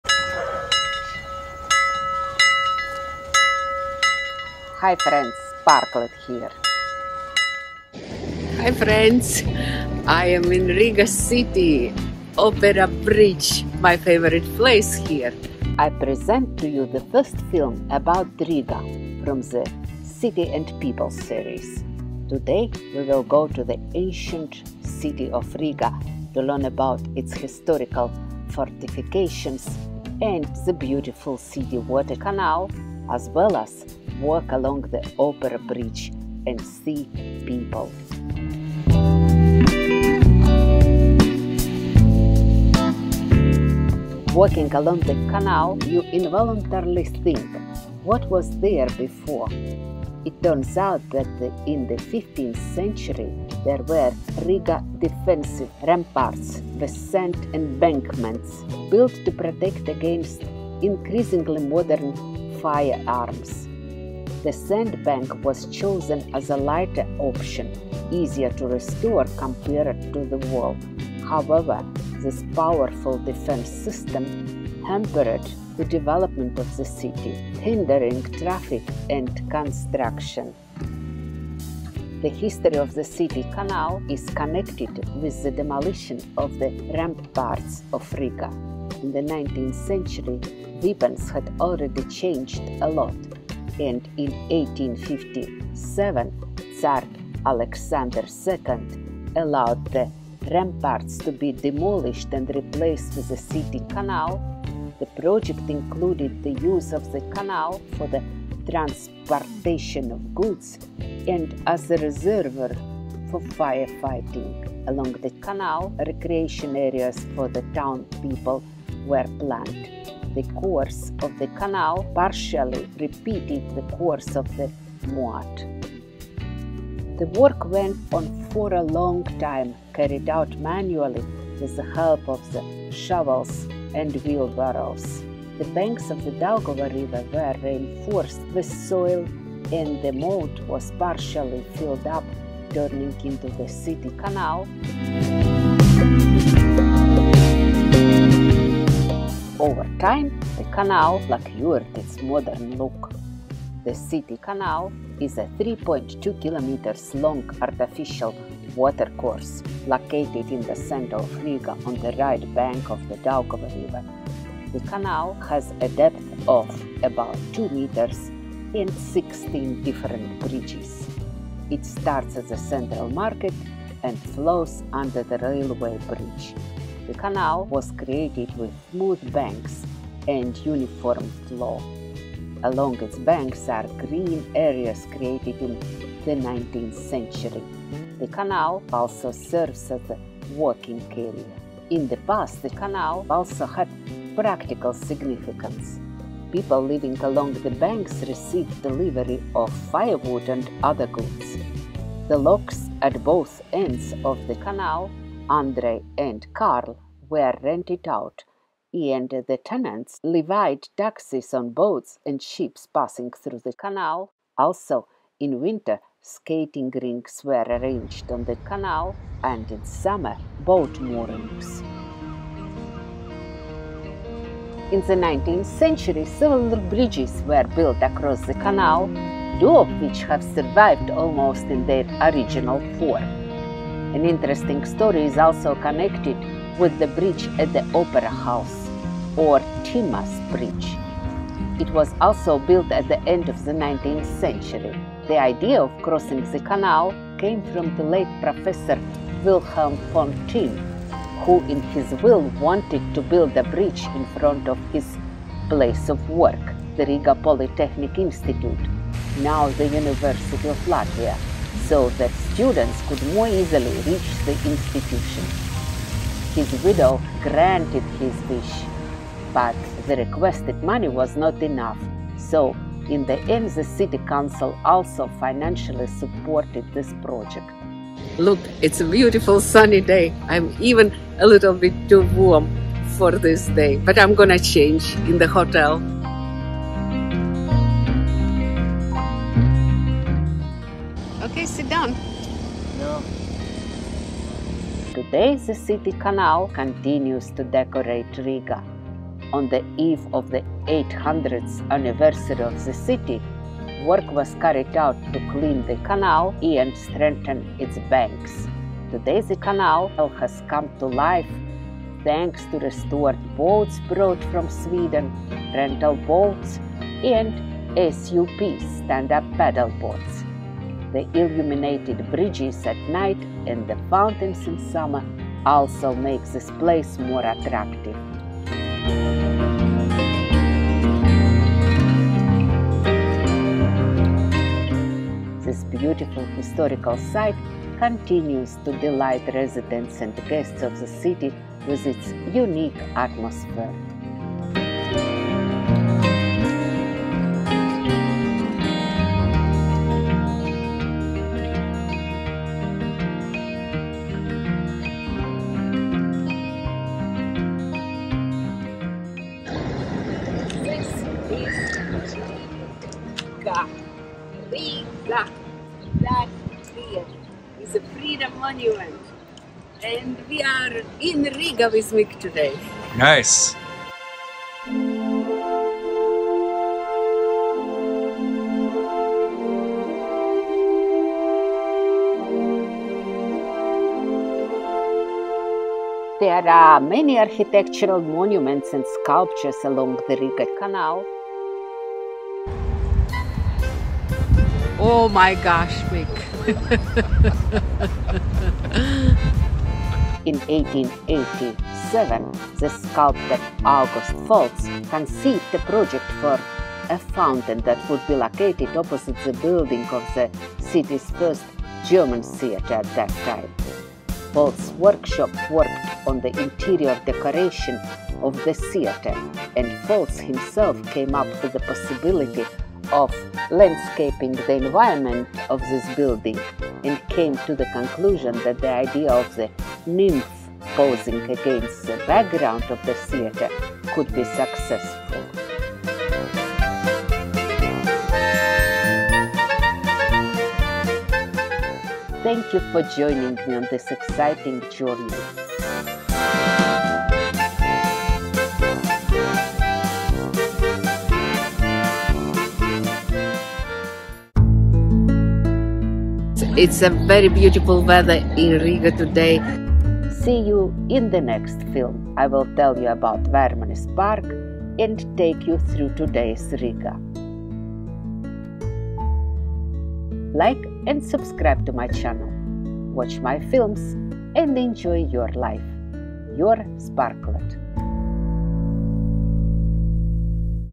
🔥 Riga's Historical Secrets Unveiled: Journey Through Fortifications and Captivating City Canal
🇱🇻 Welcome to the first film of the captivating "City and People" series, featuring the enchanting city of Riga. Join me as we embark on a journey through time, exploring Riga's historical fortifications, the mesmerizing city water canal, and strolling along the magnificent opera bridge, all while immersing ourselves in the vibrant atmosphere of this remarkable city.
0:00 Start
0:07 Timma Opera Bridge in Riga
0:31 The first film about Riga from the "City and People" series
0:37 Historical fortifications and city canal in Riga
1:37 History of the Riga City Canal
2:48 Riga city canal with 16 bridges
3:26 Commercial value of the channel
4:00 Wilhelm Timm's Bridge at the Riga National Opera
4:58 Riga City Canal today: boats, lighting of canal bridges, walks, rest
6:04 RIGA sign and the Freedom Monument near the Riga Canal
6:30 Architectural monuments and sculptures along the Riga Canal
6:44 Fountain "Nymph" (sculptor August Filz) near the canal and the Riga National Opera
7:37 In the next film we go to Vermanis Park and walk around Riga in June

In this video, we delve into the rich history of Riga's fortifications, starting with the defensive ramparts that once stood in the 15th century. These robust sand embankments were constructed as a defense against advancing firearms, albeit impeding the city's growth and development due to traffic congestion and construction restrictions.

Discover the fascinating tale of the Riga City Canal, intimately intertwined with the demolition of the ramparts during the 19th century. With the evolution of weaponry, Tsar Alexander II granted permission in 1857 to replace the ramparts with a city canal, serving both as a transportation route for goods and a reservoir for firefighting purposes. The canal's course followed the path of the former moat, bringing the townspeople delightful recreation areas along its banks.

Witness the transformation of the Riga City Canal, a 3.2 km long artificial watercourse on the right bank of the Daugava River. Adorned with 16 picturesque bridges, the canal meanders from the Central Market to the iconic Railway Bridge. Originally constructed manually with shovels and wheelbarrows, the canal's banks were reinforced with earth, gradually taking on its modern appearance.

Once an essential lifeline for the local inhabitants, the canal facilitated the delivery of goods and levied taxes on passing boats and ships. The locks at both ends, Andrey and Karl, played pivotal roles in maintaining the canal's functionality. During winter, the frozen canal transformed into a beloved skating rink, while summer brought joy with its lively boat moorings.

Admire the architectural splendors and sculptures adorning the canal's pathway, showcasing the city's rich heritage. Notably, the Timma Opera Bridge, constructed in the late 19th century, carries a captivating story. Conceived by the visionary Professor Wilhelm von Timm, the bridge fulfilled his desire to ease students' access to the Riga Polytechnic Institute. With the widow's support and the city council's financial assistance, the bridge became a testament to Timm's legacy.

Today, the Riga City Canal thrives, offering delightful experiences for locals and visitors alike. The canal has been revitalized with restored boats from Sweden, as well as rental boats and SUP activities. The illuminated bridges illuminate the night, while the enchanting fountains enhance the allure of this historical gem.

Immerse yourself in the unique ambiance as you explore the canal's banks adorned with architectural marvels and captivating sculptures. Noteworthy among them is the splendid nymph fountain, conceptualized by the esteemed sculptor August Foltz. Situated opposite the city's first German theater, this exquisite fountain captures the essence of beauty and harmony.

Thank you for joining me on this enthralling journey through Riga's historical fortifications and the magnificent city canal.

Let's make the most out of life together! Your host, 🔥Sparklet!